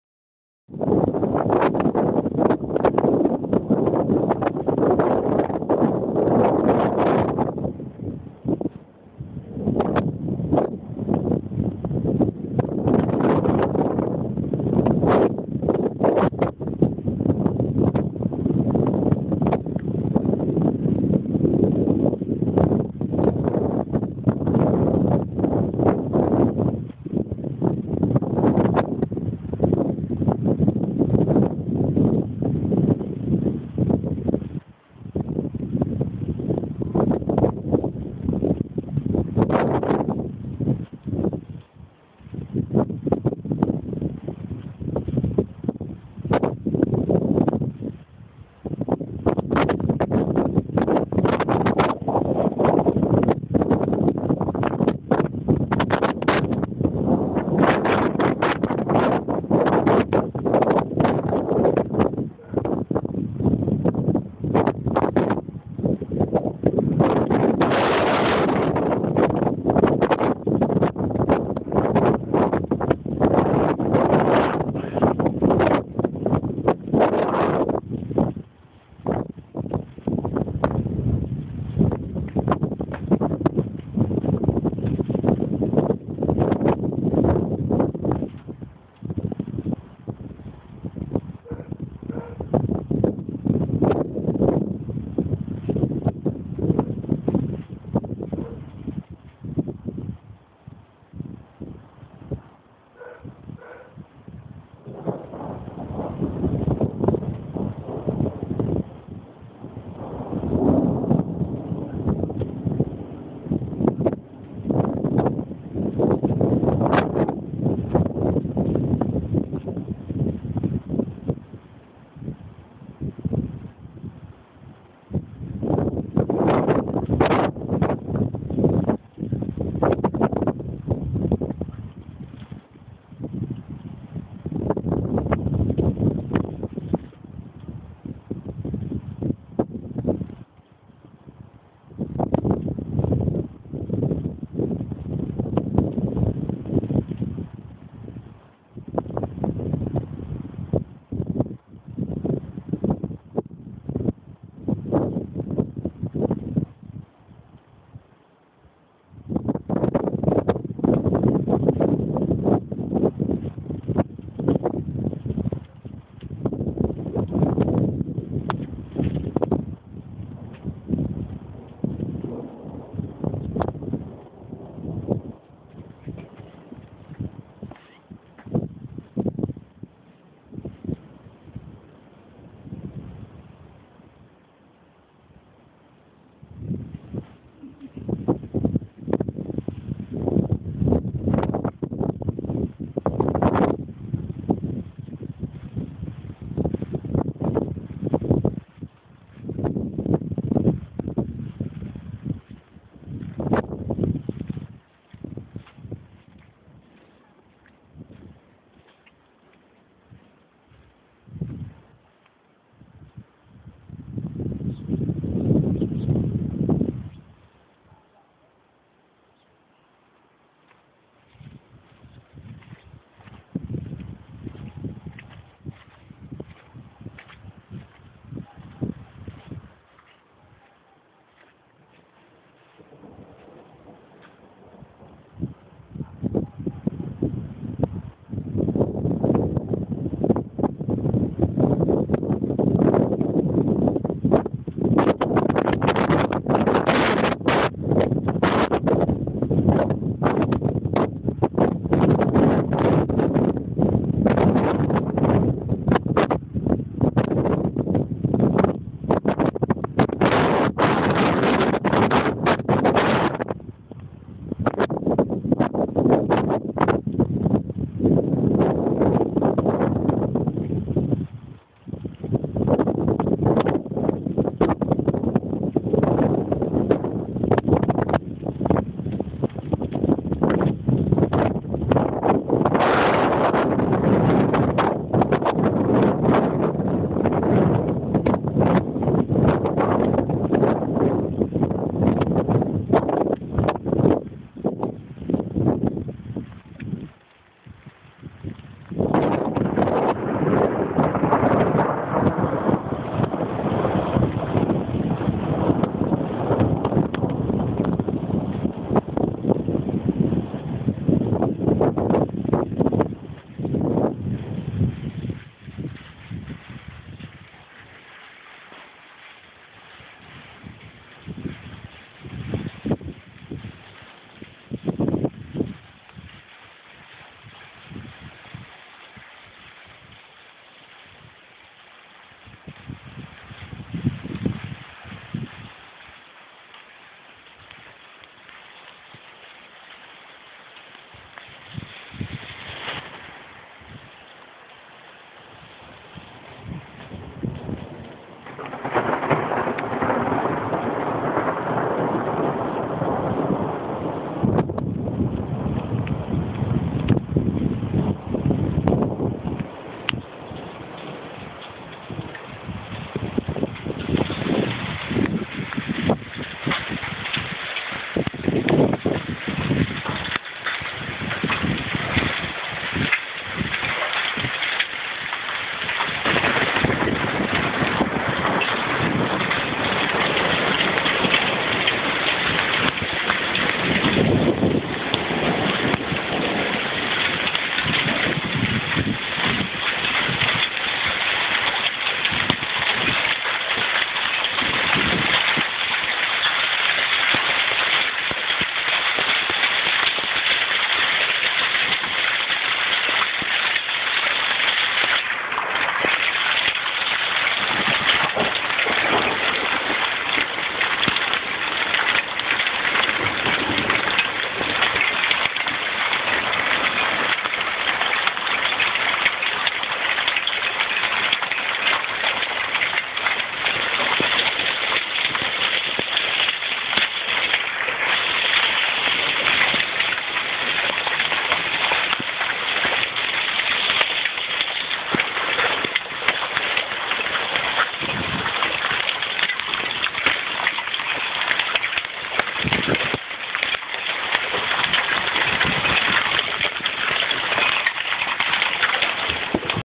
Led - Ice -.U susret padanju leda i kise... - Meet the falling ice and rain
Snimao sam oblake,sunce koje oni skrivaju,igru munja i groma prije tuce... iz moga dvorista

Meet the falling ice

I shot the clouds, the sun that they hide, play lightning and thunder before the downpour of ice ... from my courtyard